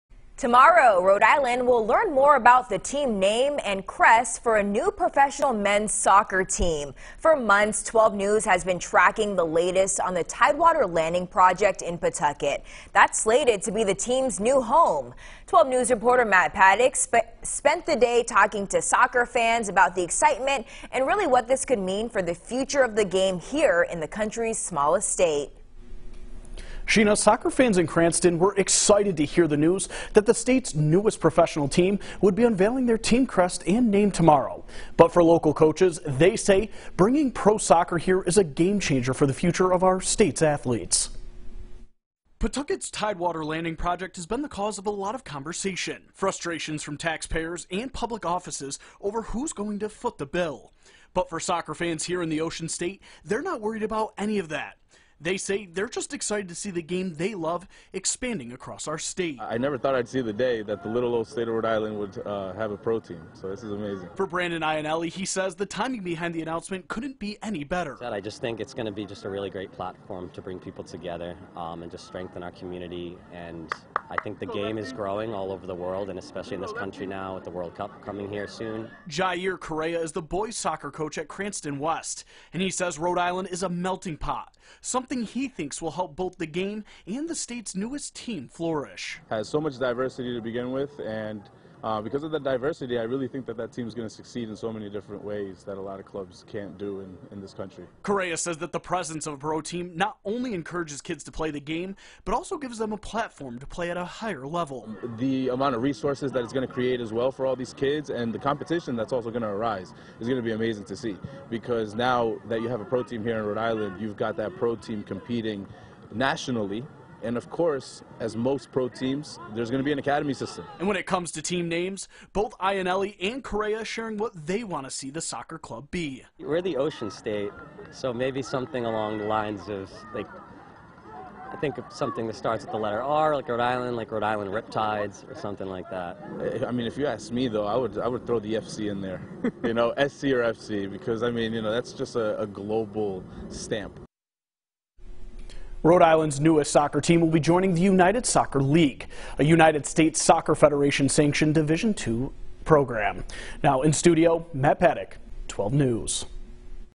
RI soccer team to unveil name and logo Monday
The team will be playing their home games at the new stadium being built in Pawtucket as part of the Tidewater Landing Project.